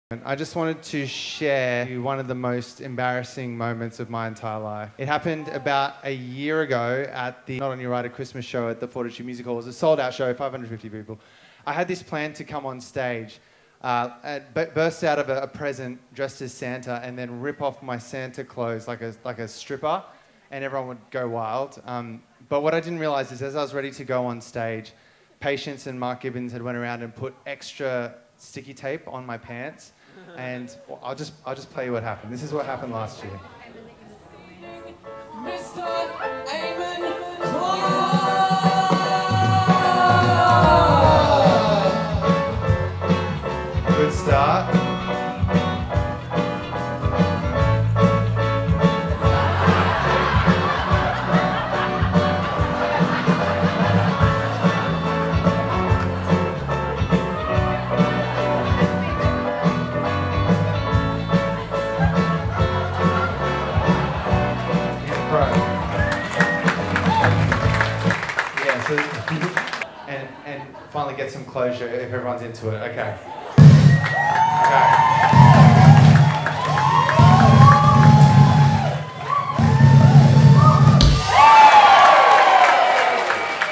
Aimon Clark's Most Embarrassing Moment
Aimon shares one of his most embarrassing moments at the intro for our Xmas show and seeks redemption.